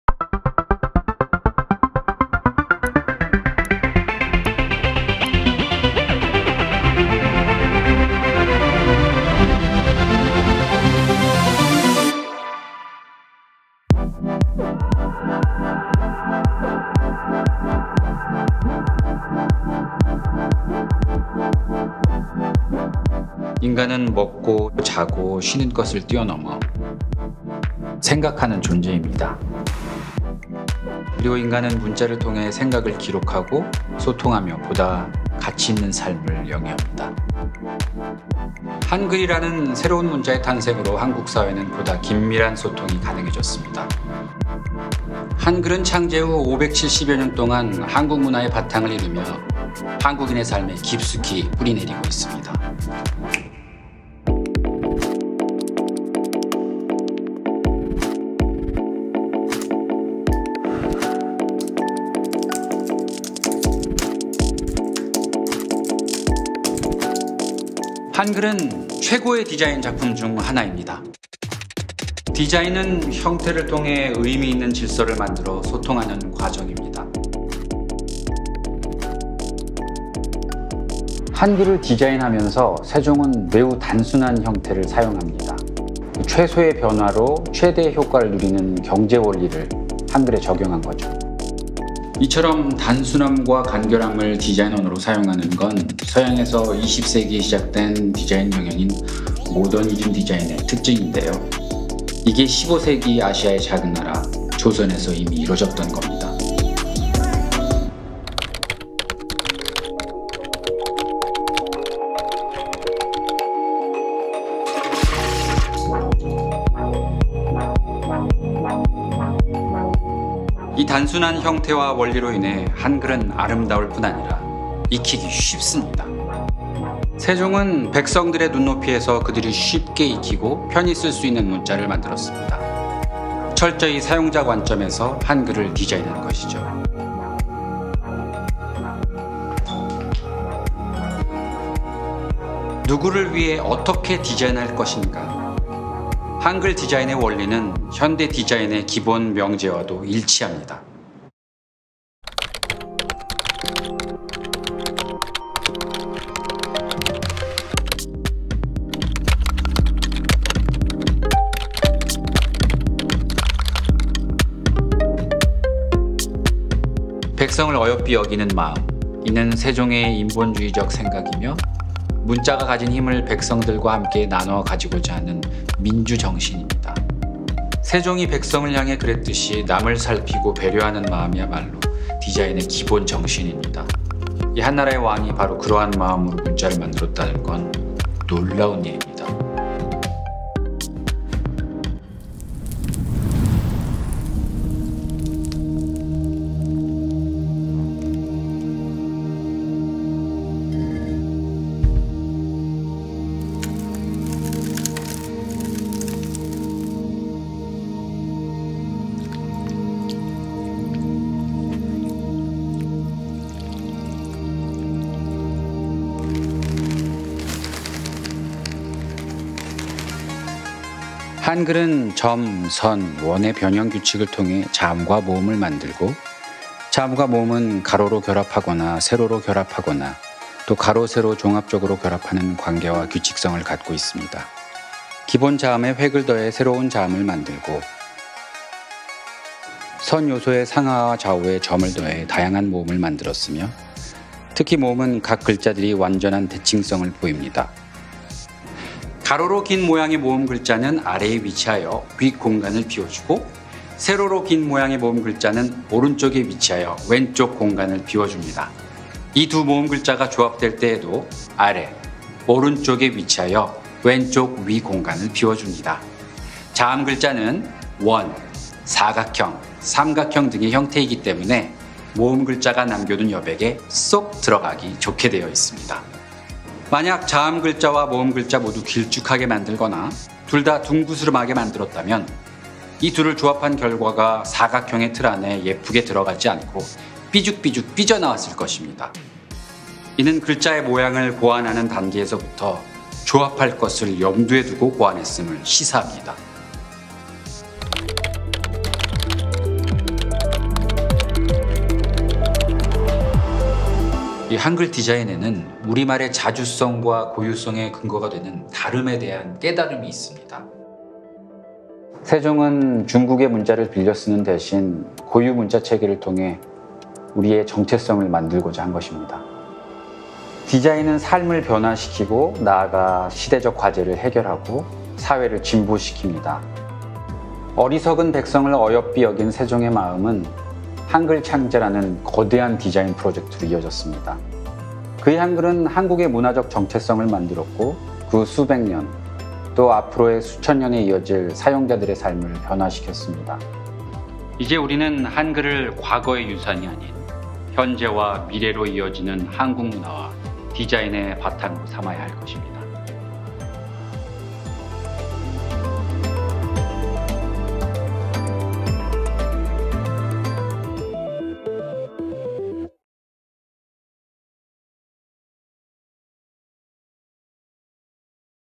[국립한글박물관] 제3회 한글실험프로젝트 '한글디자인: 형태의 전환': 지금, 여기, 우리의 한글
지금, 여기, 우리의 한글

조선의 크리에이터 세종. ‘훈민정음’ 창제는 거대한 디자인 작업이다.
훈민정음 안에는 오늘날에도 통용될 수 있는 디자인 철학이 담겨 있다.

훈민정음에 나타난 주체미학정신, 실용미학 정신, 감응미학 정신에
주목해 왜 한글을 위대하다고 칭하는지,
어떻게 자주성과 고유성을 갖게 되었는지 알아본다.

제3회 한글실험프로젝트 '한글디자인: 형태의 전환'
2019.9.9.~2020.2.2.